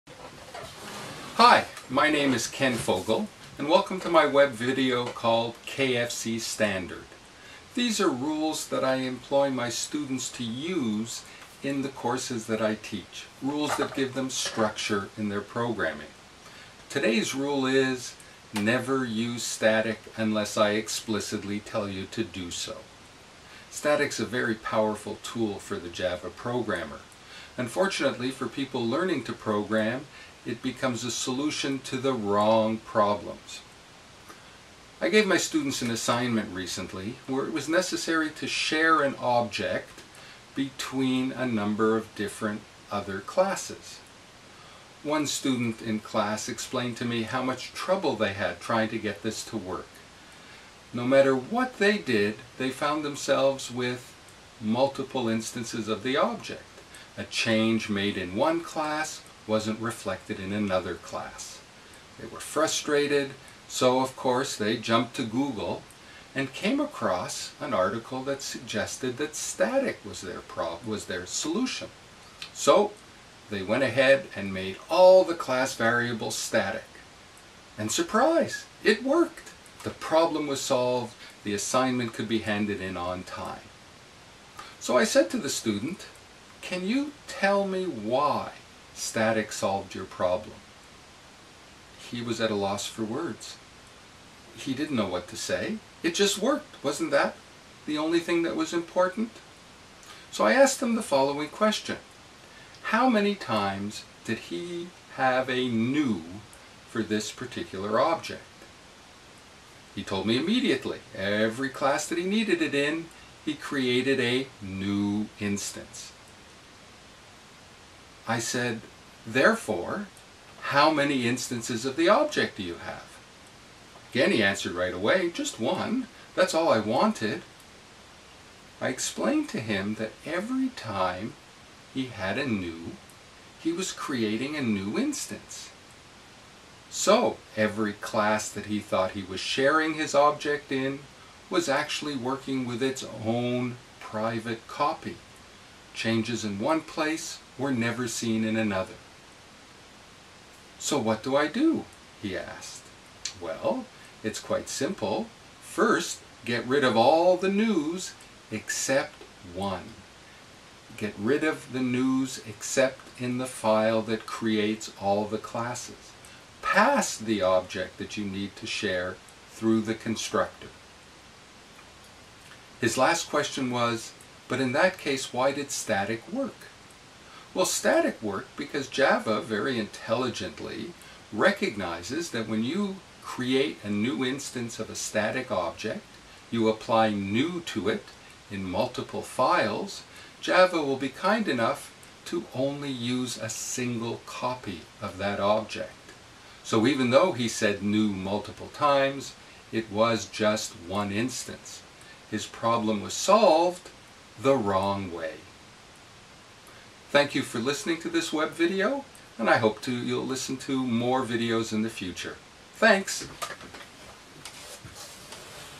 KFCStandard - Never use static in Java unless explicitly told to do so.
This is the first of my KFCstandard videos. It was created as a sample of my presentation style for a JavaOne 2015 presentation I am proposing. It is not the only topic of my presentation but one of many I hope will engage my audience with to discuss what students need to learn. I also hope that this will be the first of a series of Ken Fogel Coding standards that I use in my classes.